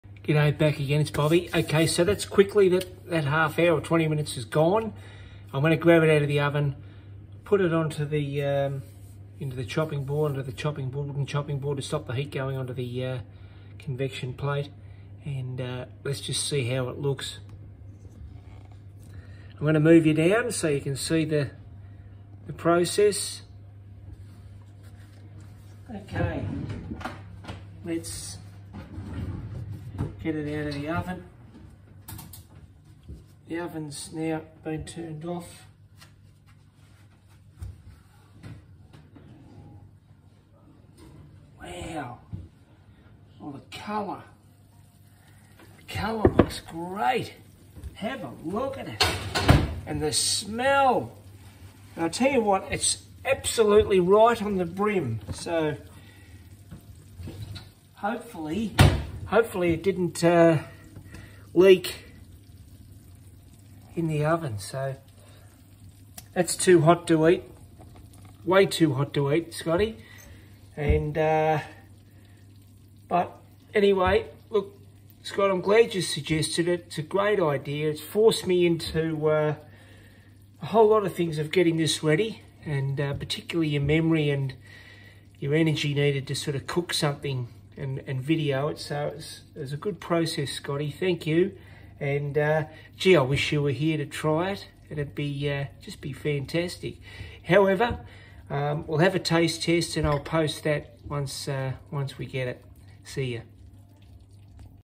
Shepherds Pie cooked
Breadcrumbs and tasty cheese is the clincher.

As I carefully dragged the pie from a hot oven the crusty hard shell like cover was golden brown and beneath was the most beautiful tasting meat pie sauce - perfect. This samples my first plate this was clearly the best meal I had cooked for some time.